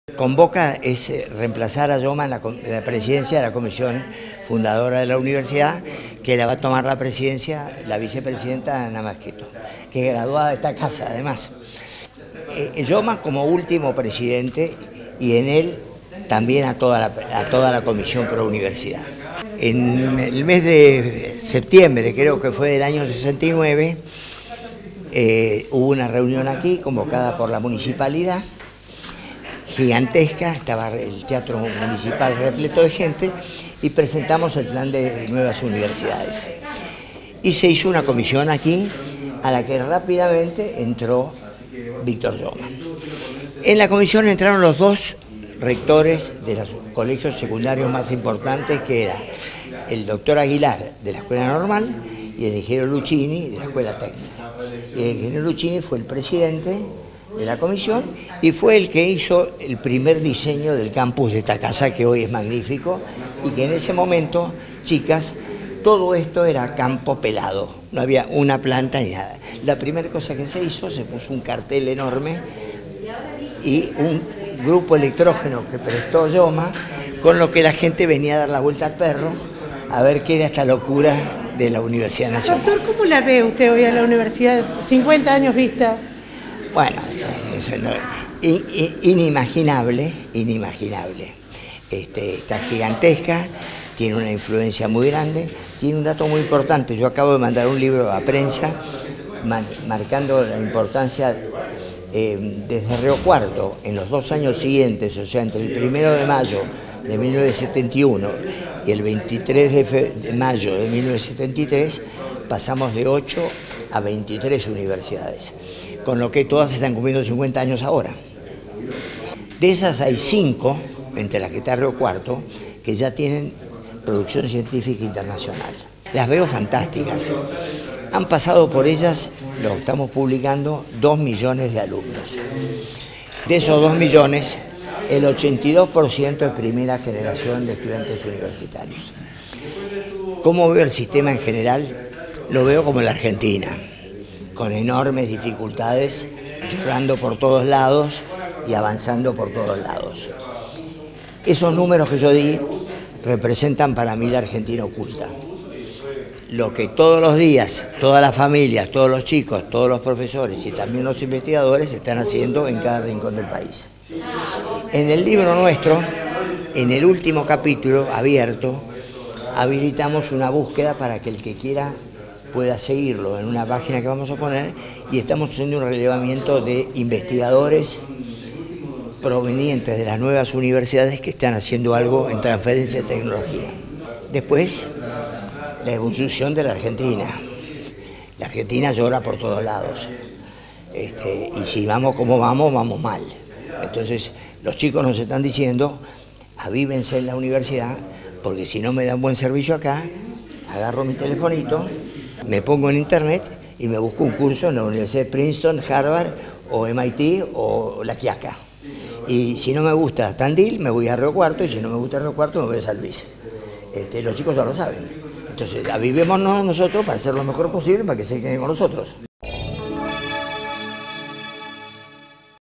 Visita de Alberto Taquini a la UNRC
Alberto Taquin - Autor Plan Nuevas Universidades
En el marco de los 50 años de la UNRC fue recibido por autoridades universitarias
Facebook.com/uniriotv
Twitter.com/uniriotv